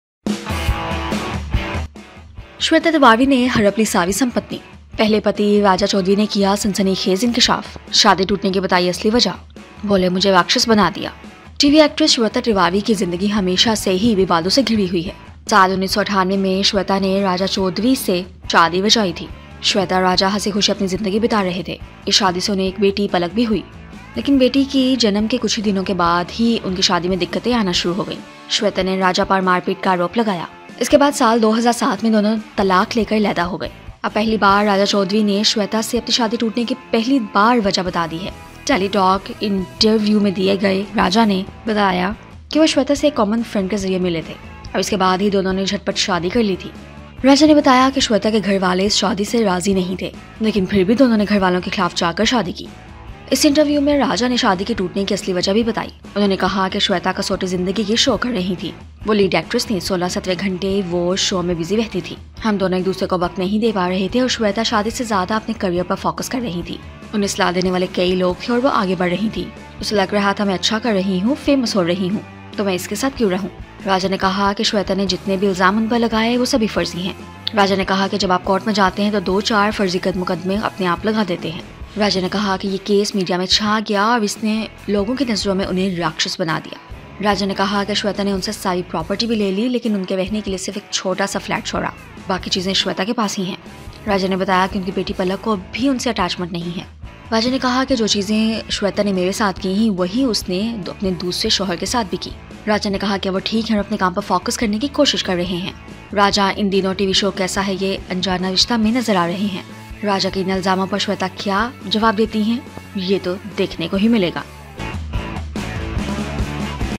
Shweta Tiwari's Ex Raja Chaudhary Charged Her For Grabbing All His Property
श्वेता तिवारी के पूर्व राजा चौधरी ने उन पर उनकी सारी संपत्ति हड़पने का आरोप लगाया